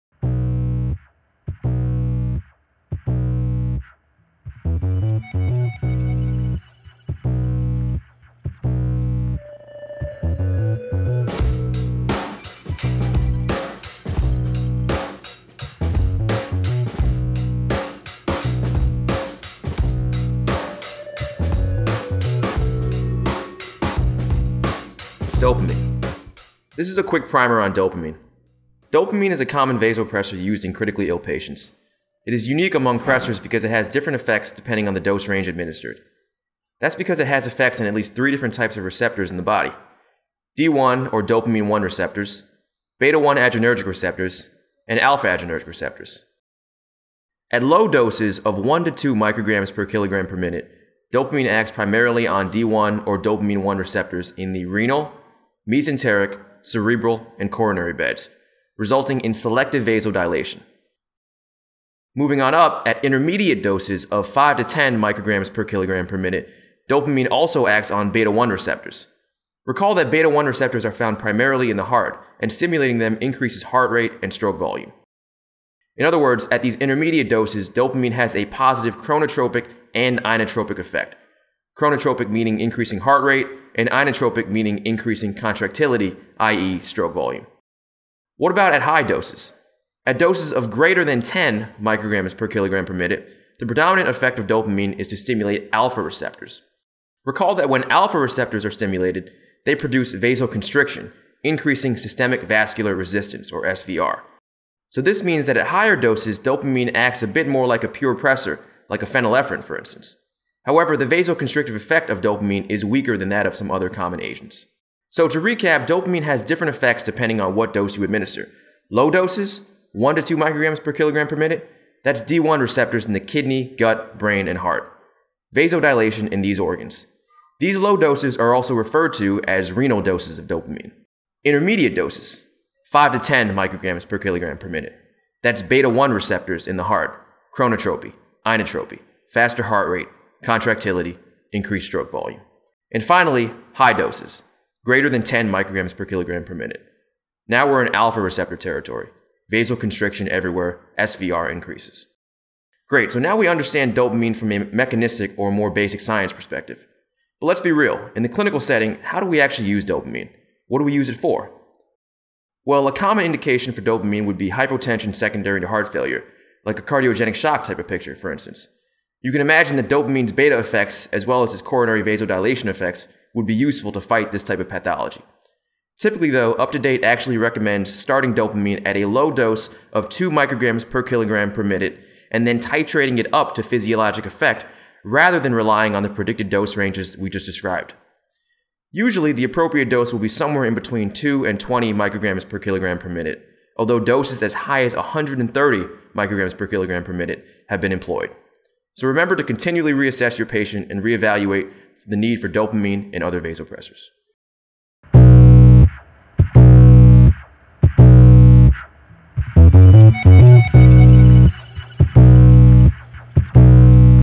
Dopamine - Vasopressors & Inotropes - MEDZCOOL
Dopamine is a complex vasopressor and inotrope that acts on many receptors in the human body in a dose dependent fashion. Learn more about dopamine here.